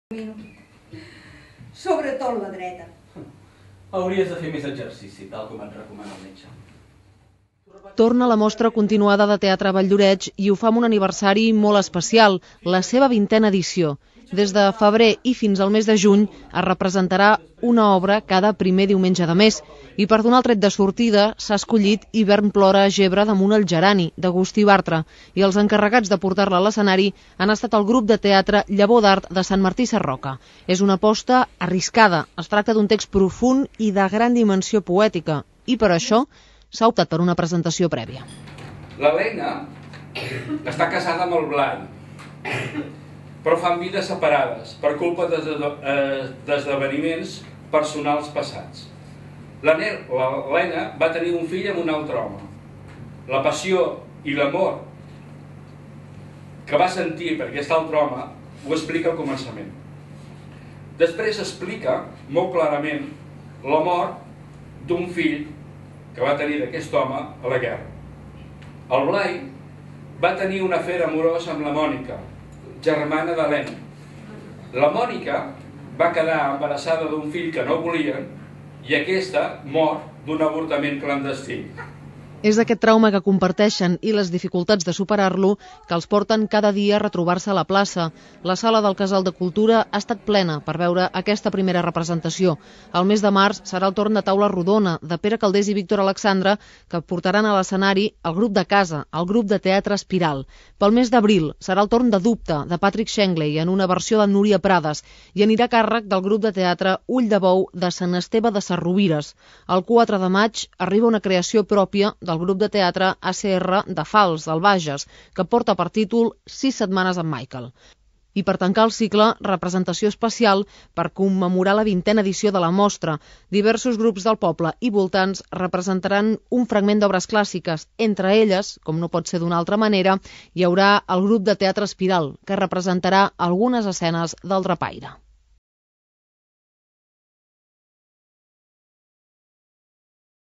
Presentació de la Programació Continuada de Teatre de Valldoreix 2014
A principis de l'any 2014 el grup de Teatre Espiral va donarel tret de sortida a la primera obra de la programació continuada de teatre que puja a l'escenari valldoreixenc una obra de teatre al mes  fins al juny.